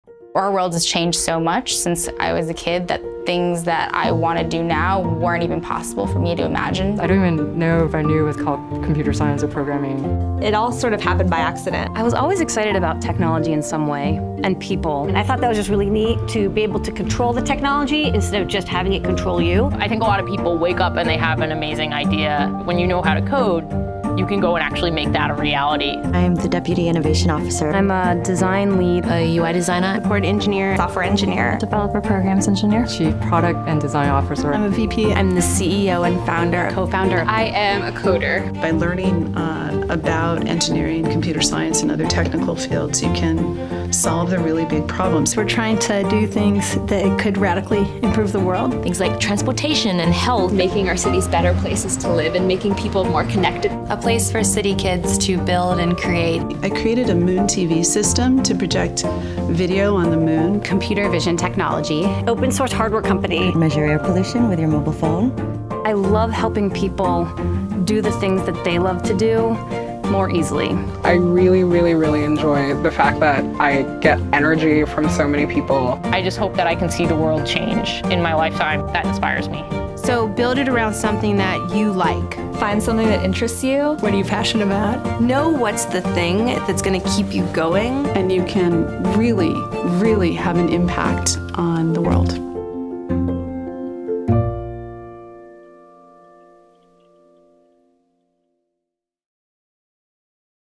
Women Techmakers: Make your passion.
We're all driven by passion. Hear from innovative women leaders as they share their personal stories, solutions, and impact on the technology industry. Featuring:

Suzanne Xie (Founder and CEO of Hullabalu)
Jeannie Yang (Chief Product and Design Officer at Smule)
Kelly Ellis (Software Engineer at Google)
Brynn Evans (Design Lead at Google)
Limor Fried (Lead Engineer and Founder at Adafruit Industries)
Ashley Gavin (Director of Curriculum at Girls Who Code)
Shannon Spanhake (Deputy Innovation Officer for the City of San Francisco)
Joanna Smith (Developer Programs Engineer at Google)
Mary Lou Jepsen (Head of Display Division at Google[x])
Megan Smith (Entrepreneur at Google[x])
Jay Wong (UI Designer and Prototyper at Google)
Bandana Malik (Student at The Flatiron School)
Jenny Young (Owner of Brooklyn Robot Foundry)
Heidi Zak Spector (Co-Founder at ThirdLove)
Aminatou Sow (Founder of Tech LadyMafia)
Cassandra Dixon (Support Engineer at Fastly)
Natalie Gordon (CEO and Founder at BabyList)

Visit g.co/WomenTechmakers for more information about the Women Techmakers program.